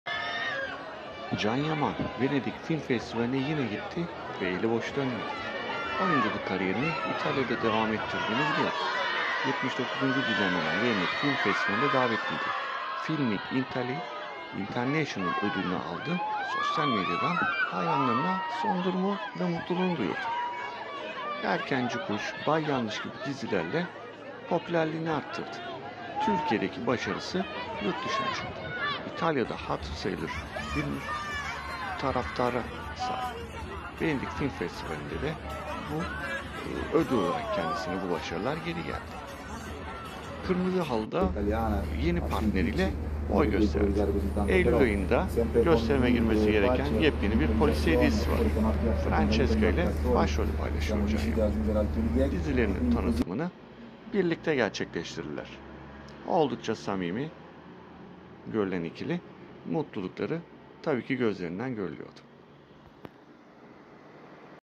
CAN YAMAN YENİ DİZİ TANITIMINDA KIRMIZI HALIDA
Can Yaman & Francesca Chillemi today from the Cosmopolitan event

Viola Come İl Mare fragman , Viola Come İl Mare series,